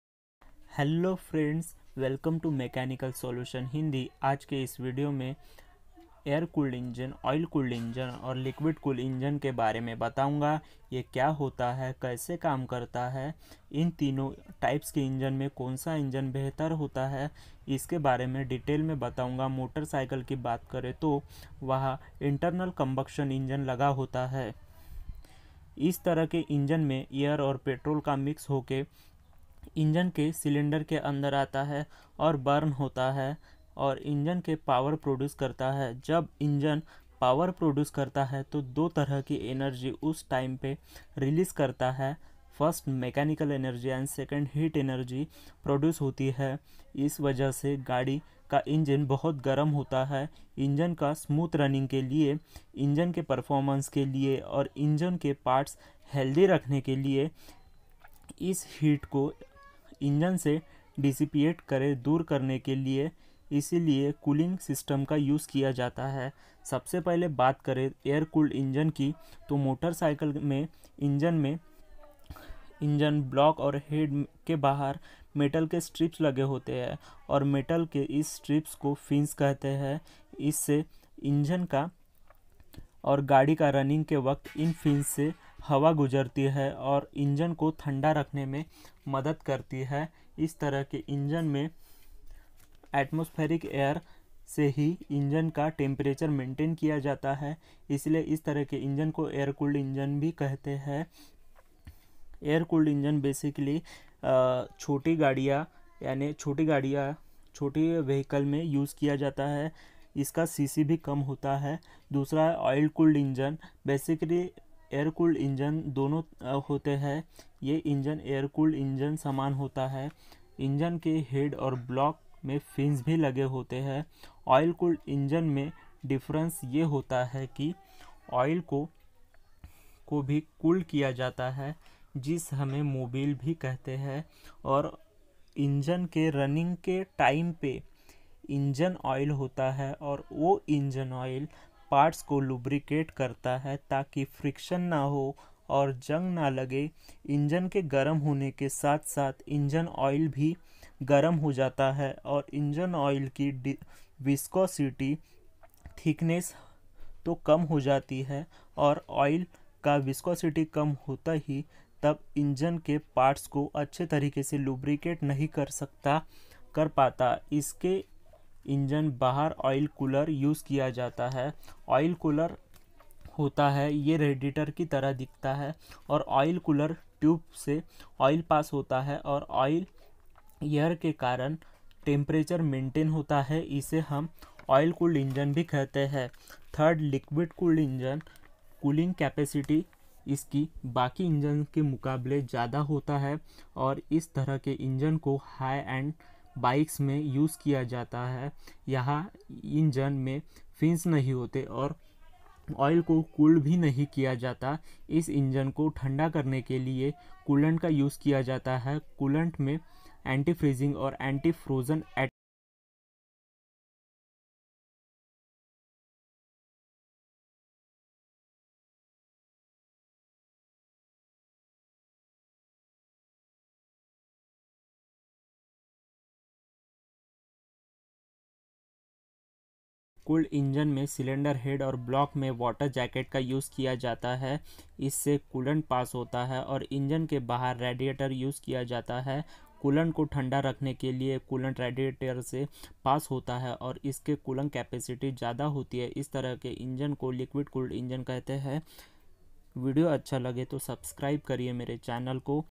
Air cooled Vs Oil cooled Vs Liquid cooled Engine कौन सा सबसे अच्छा हैं? Hindi
Welcome

All mechanical community support.।।।।।।।।।।।।।।।।।।।।।।।।।।।।।।